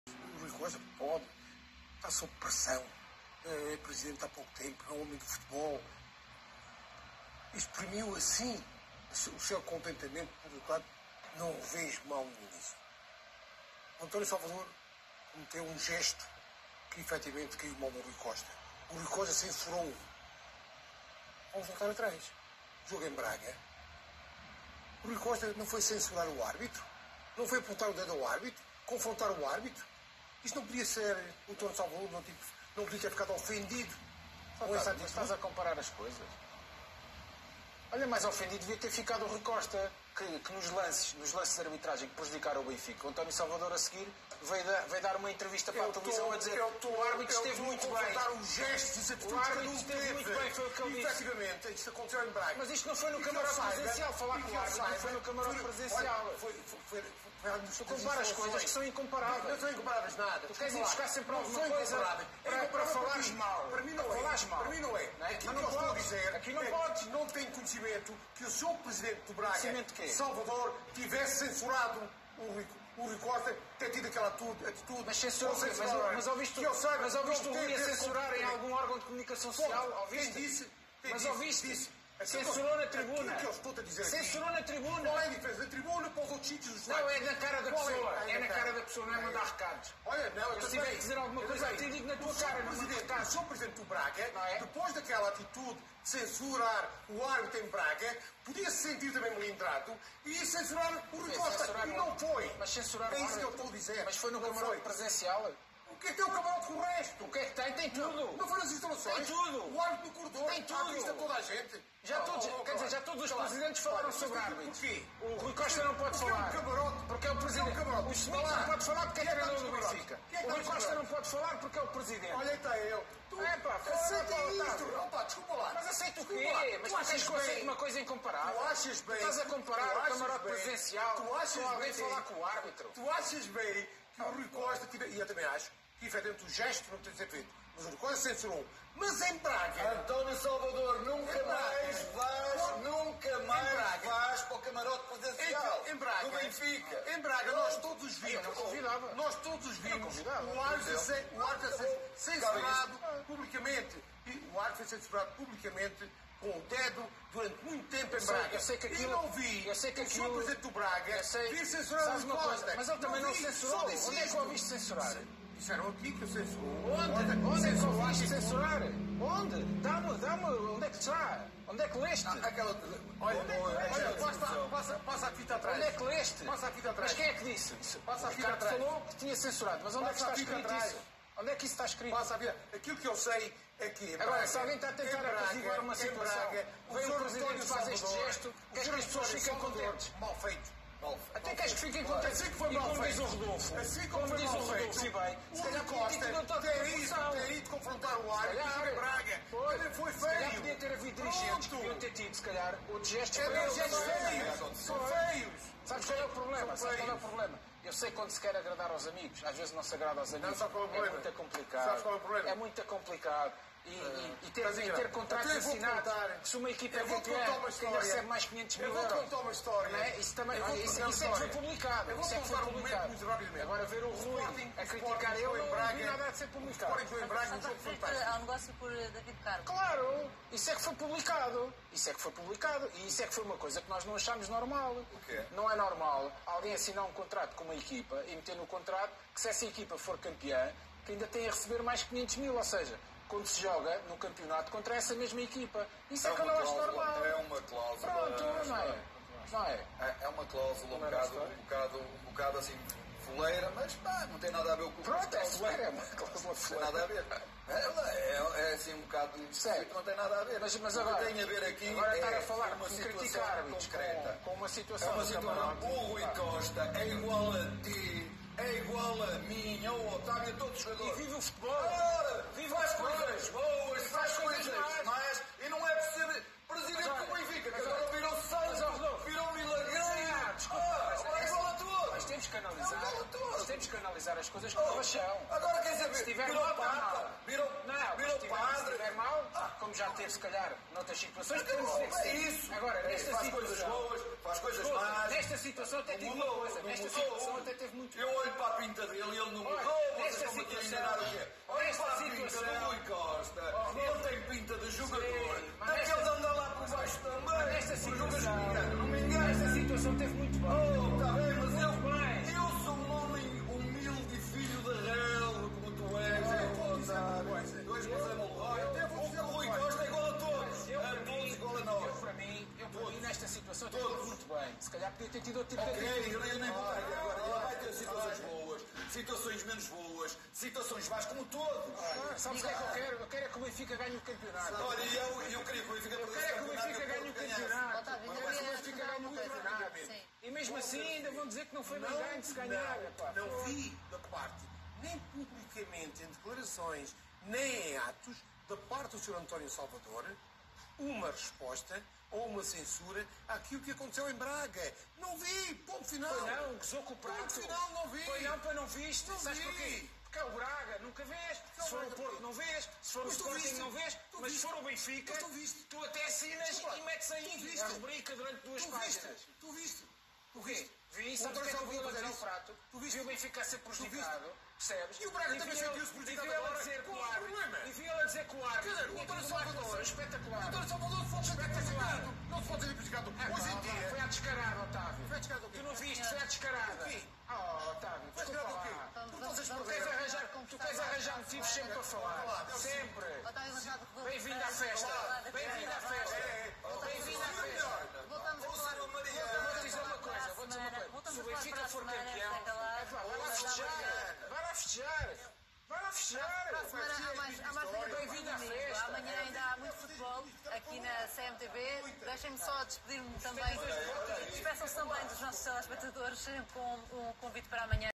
Peixeirada entre Octávio Machado e Calado na CMTV: 'Digo na tua cara' (Vídeo)
No programa desportivo desportivo da CMTV, Octávio Machado e José Calado discordaram na hora de analisar as queixas de António Salvador sobre a arbitragem do Benfica-SC Braga.

Saiba mais!